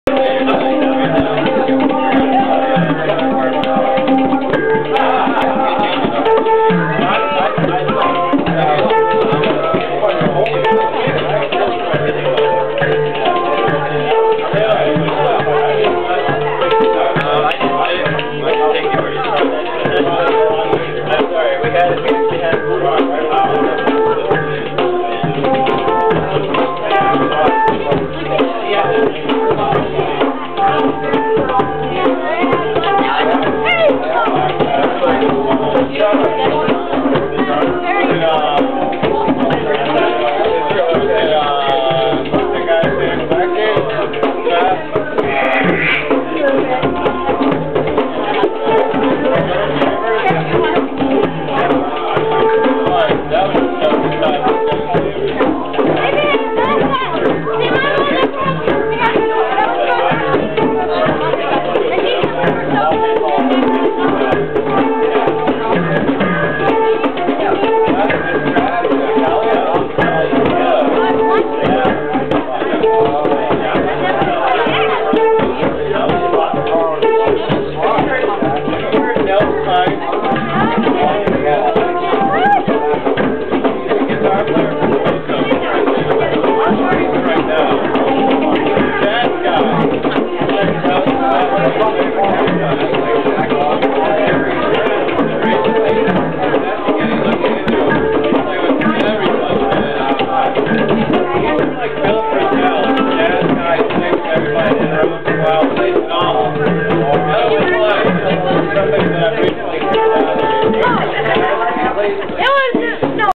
Hangin' on a Hang Drum
This was my very first time ever playing one of these drums.  SOOO MUCH FUN. The rythm just comes naturally.  It was taken at a drum circle in pittsburgh , sorry that the people in the background are loud and that the lighting is bad =(  .